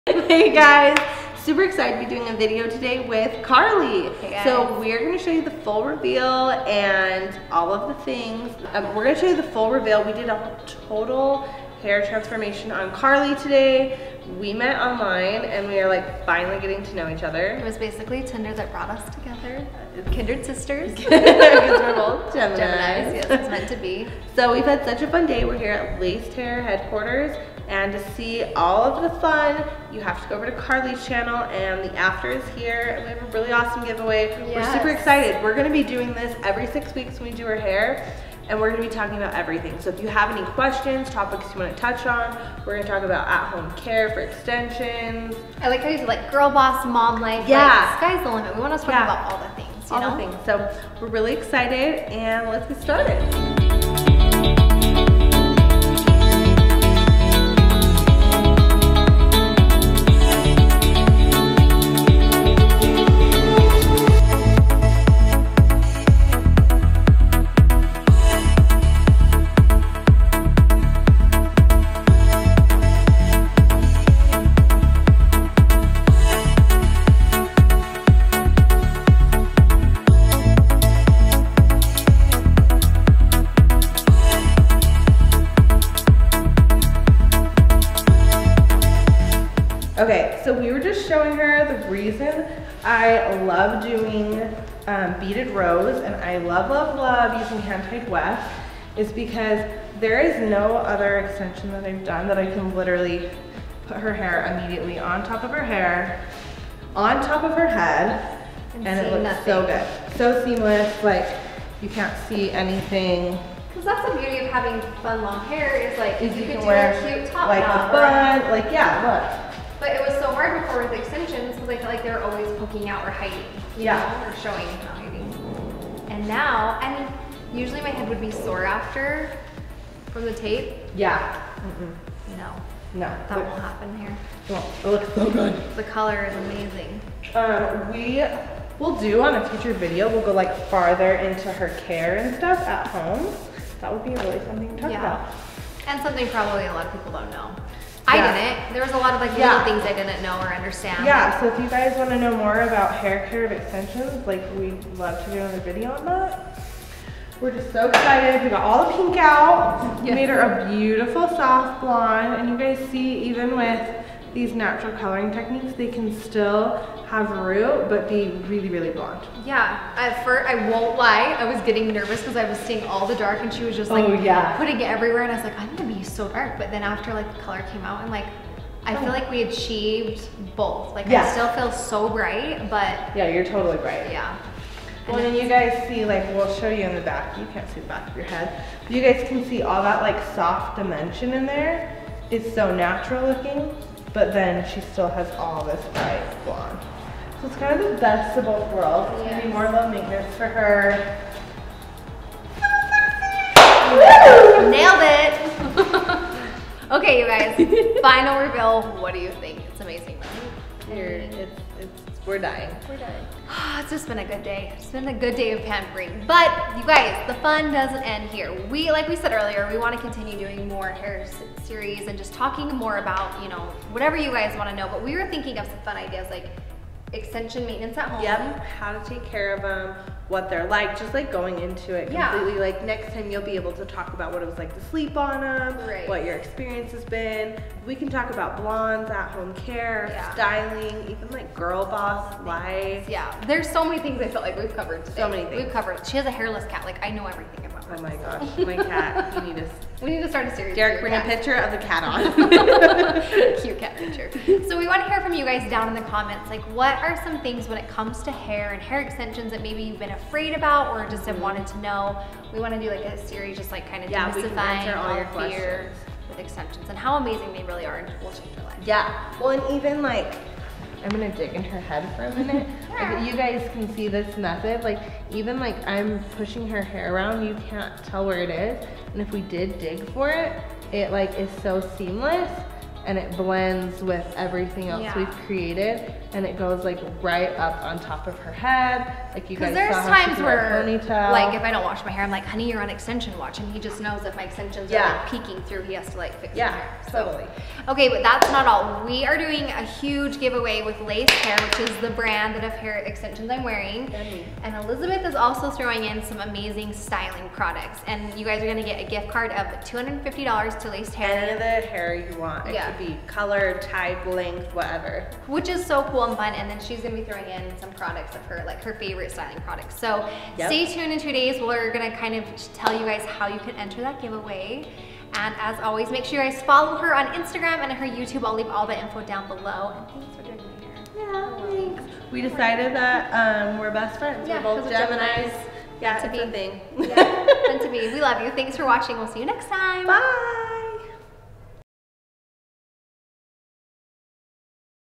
Hair Transformation with CarlieStylez - Extensions, Balayage, Blonding!
And now the reveal! @CarlyStylez and @HeyElizabethFaye team up to bring you a hair transformation and we have a lot of fun doing it! You can see the final look in this video!

I'll talk about some tips and tricks for your extensions as well in this video and explain why I love beaded rows and hand tied wefts.

We put in some great hand tied extensions using a beaded row technique to give Carly an invisible look with long natural flowing hair. I used my famous "pop piece" to give her some natural dimension and incorporated some balayage and smudge and tone techniques as well to give her a beautiful, sun kissed, natural look.